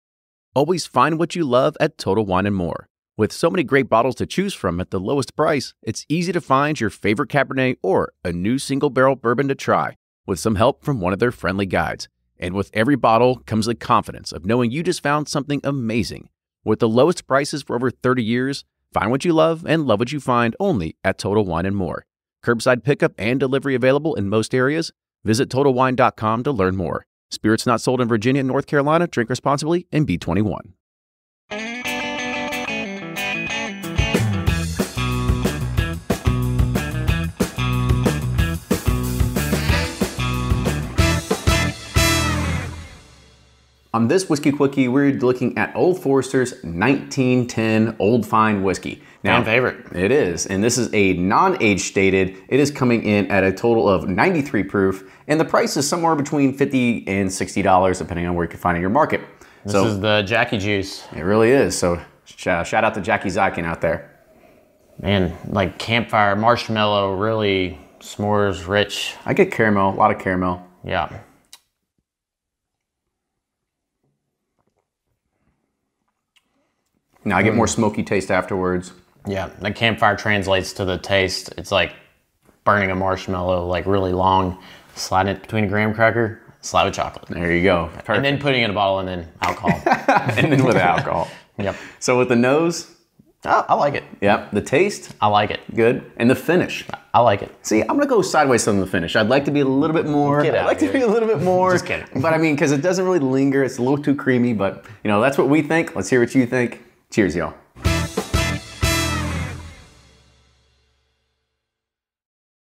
Whiskey Quickie: Old Forester 1910 Old Fine Whisky Review
On this Whiskey Quickie by Bourbon Pursuit, we review Old Forester 1910 Old Fine Whisky This non-age stated bourbon is 93 proof and $55 MSRP. Let us know what you think. Cheers!